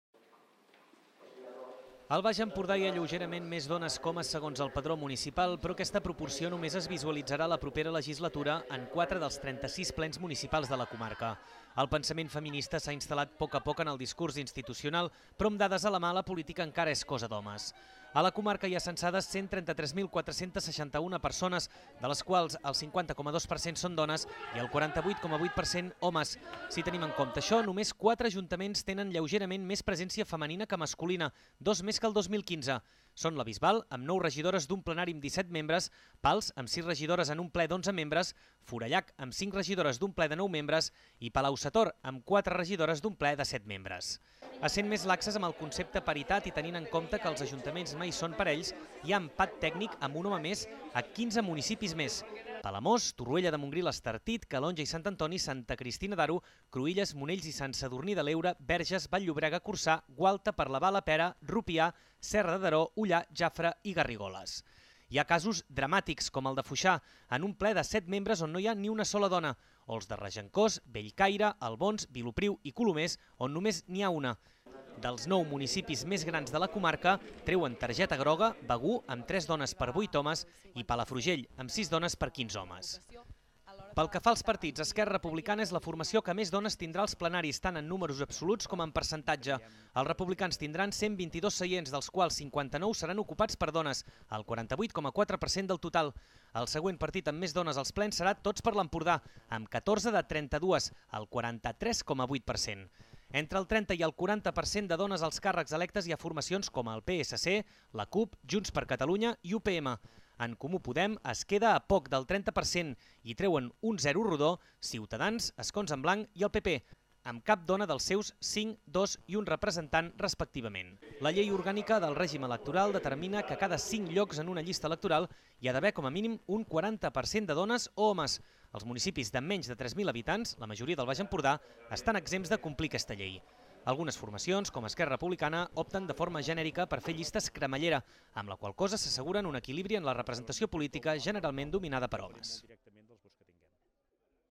Només 4 ajuntaments del Baix Empordà tenen més dones que homes al plenari
Al Baix Empordà hi ha lleugerament més dones que homes segons el padró municipal però aquesta proporció només es visualitzarà la propera legislatura en 4 dels 36 plens municipals de la comarca. El pensament feminista s'ha instal·lat poc a poc en el discurs institucional però amb dades a la mà la política encara és cosa d'homes.

Notícia emesa a TV Costa Brava el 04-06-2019